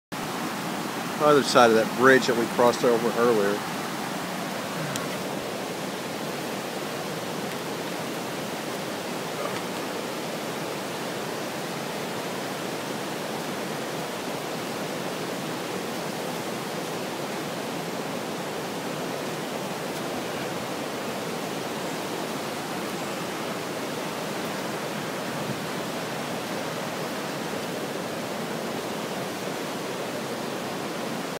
Niagara Falls 19
Crossing back to the north side of American Falls.